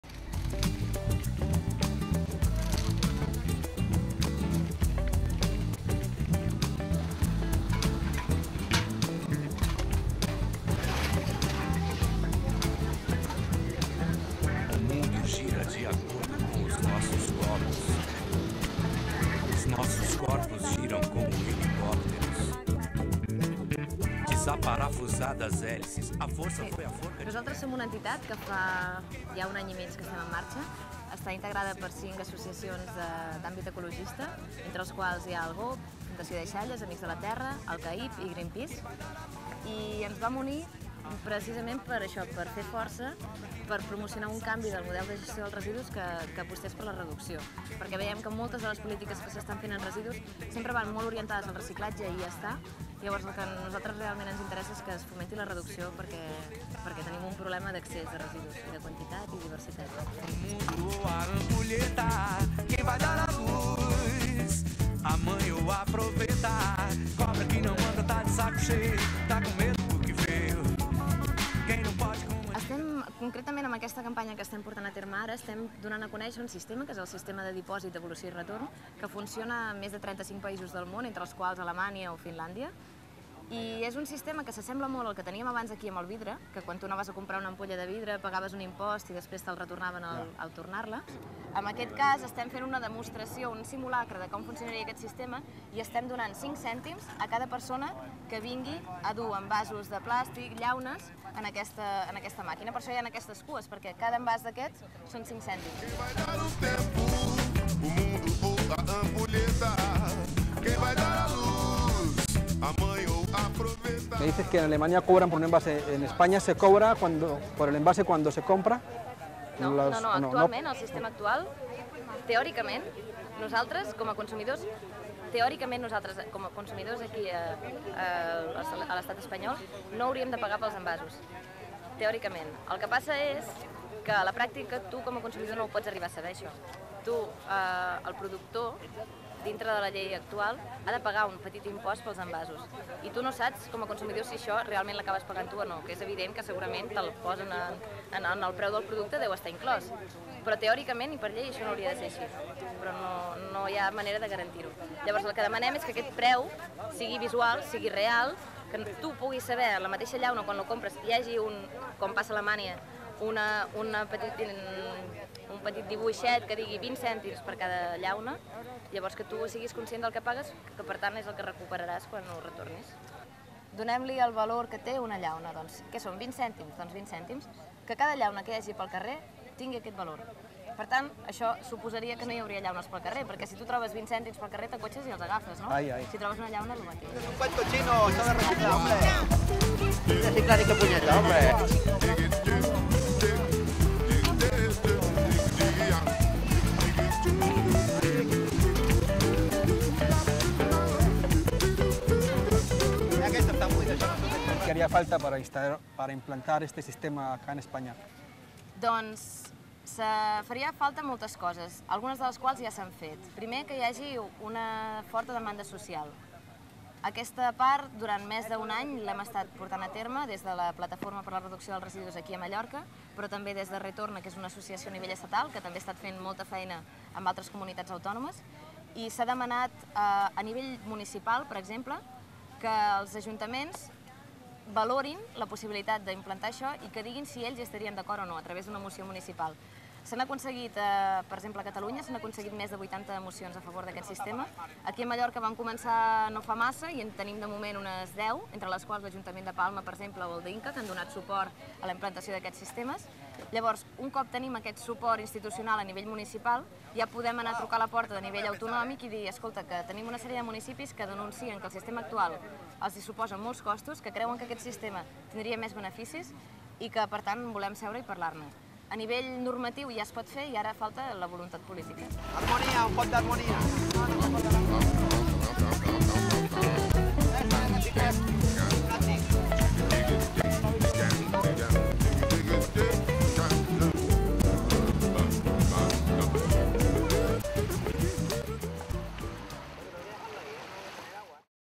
Projecte Retorna. Reutilització ecològica
Reportatge sobre el projecte Retorna que promou la reutilització. L'entitat va fer una demostració de com funcionaria el sistema de devolució d'envasos a la platja de Ciutat Jardí el passat dia 20 d'Agost amb una parada a on tothom qui volgués podia bescanviar els seus envasos per 5 cèntims. El Barrireporter César Ortiz (Veí de la zona) va estar allà per explicar-ho